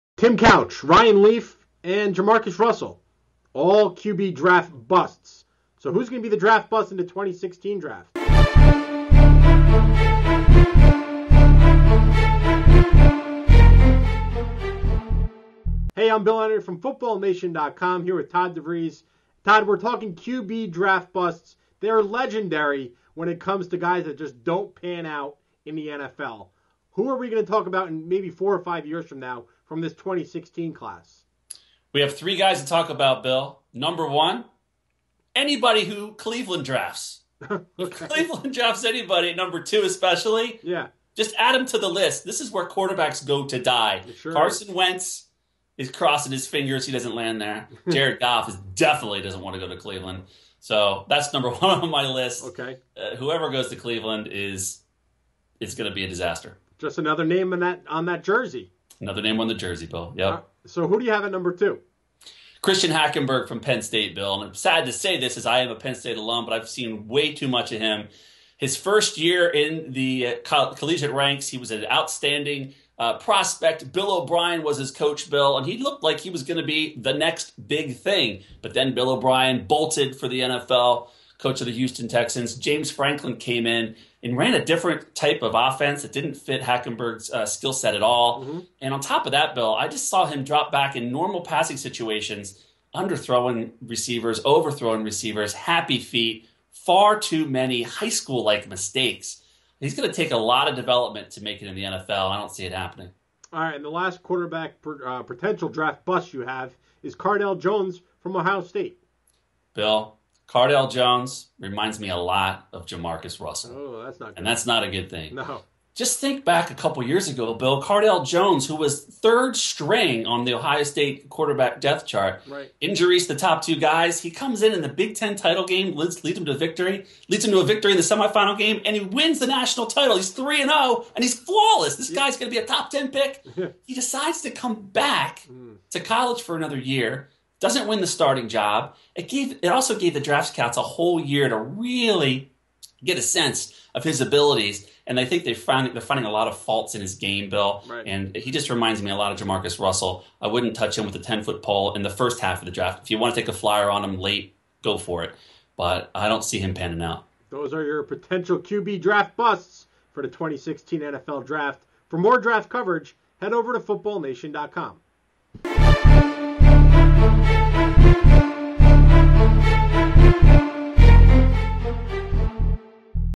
Three QB Busts In The 2016 NFL Draft
Cardale Jones and Christian Hackenberg will be the next group of quarterbacks to join the likes of JaMarcus Russell and Ryan Leaf as BUSTS in the NFL Draft. Watch the above video as Todd DeVries and Bill Enright discuss why the former Penn State and Ohio State passers won't succeed in the NFL.